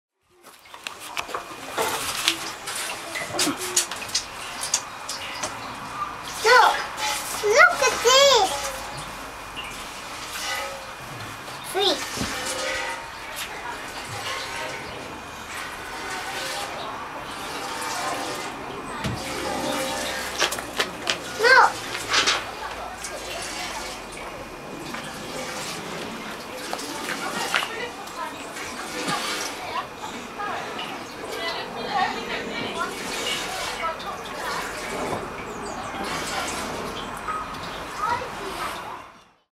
Angus Milks a Wooden Cow
Staunton Country Park, August 2010.